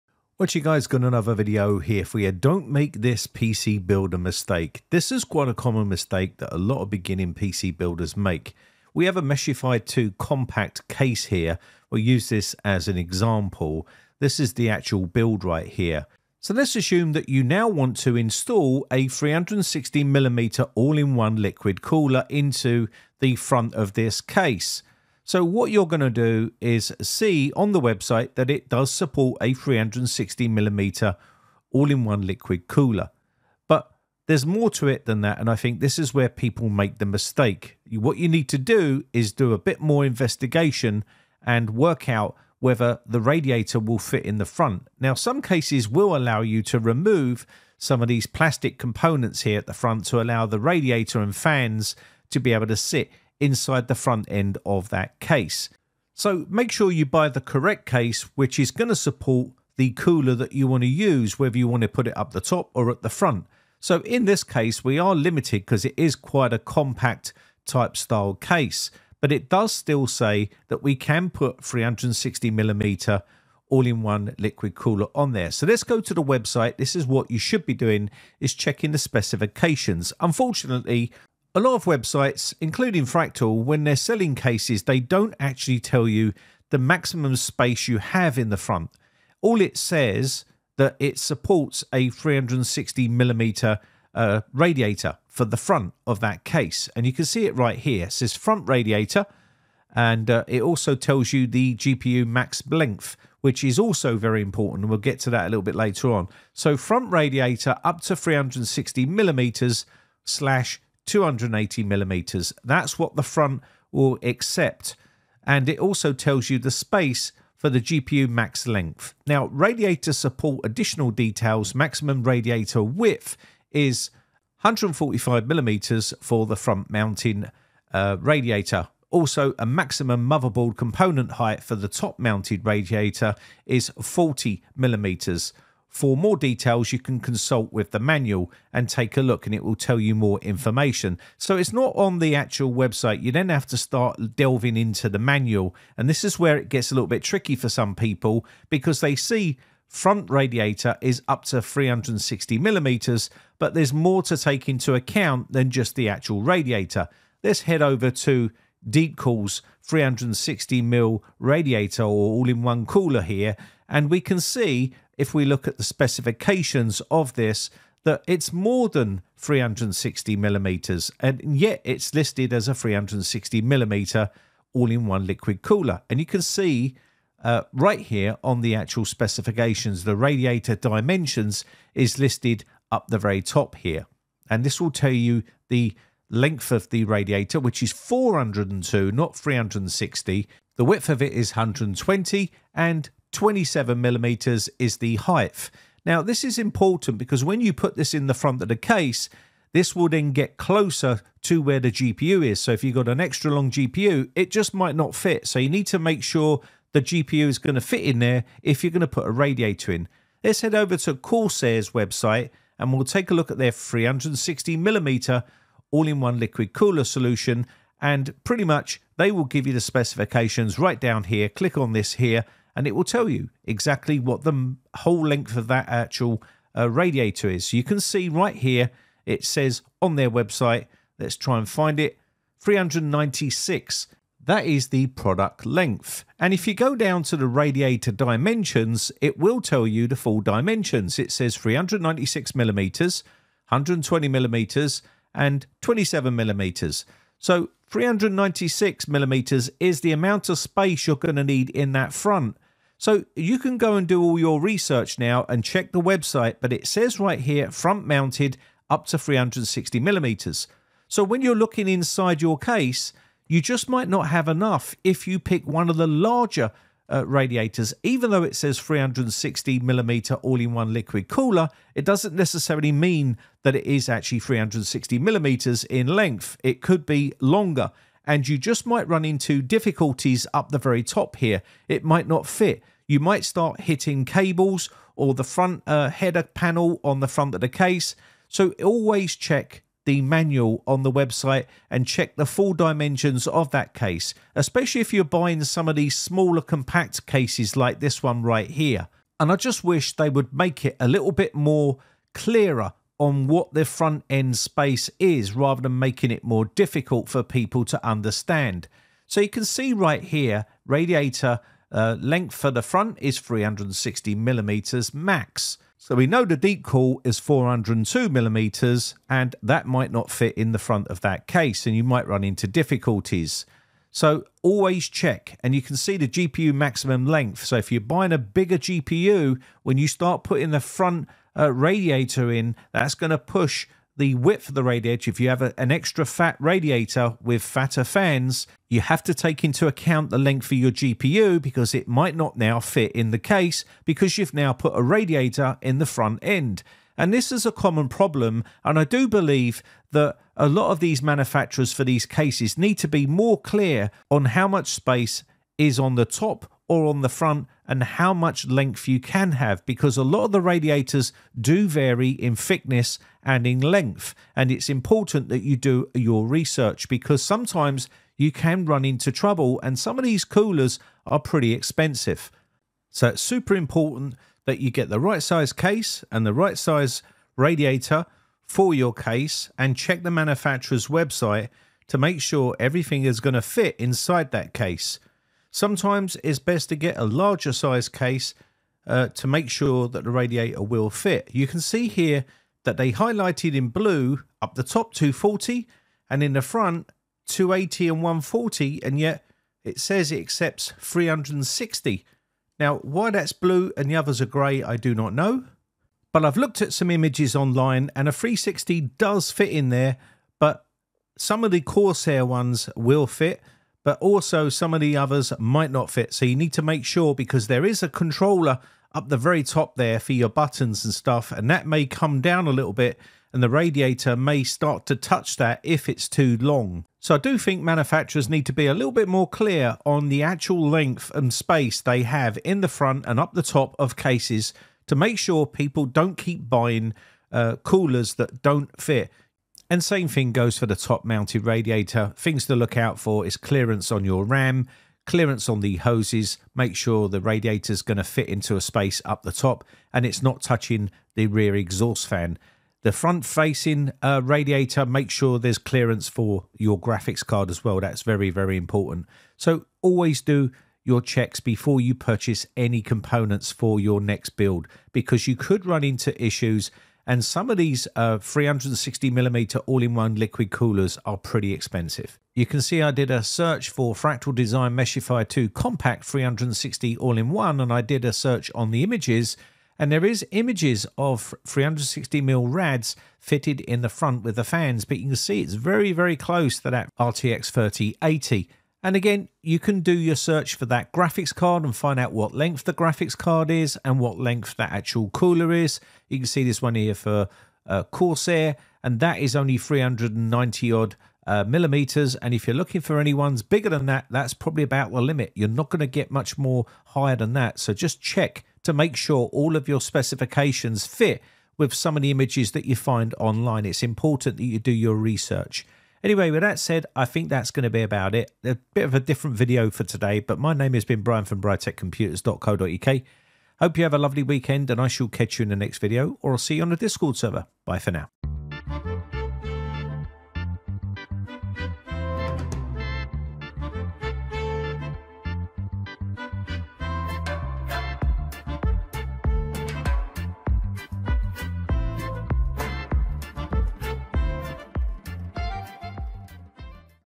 Don't Make This PC Builder Mistake
Picking the right parts is an essential part of pc building. 360mm AIO liquid coolers are very tricky if you choose the wrong size cooler, because they are longer than 360mm. So choosing the right size to fit the case is essential.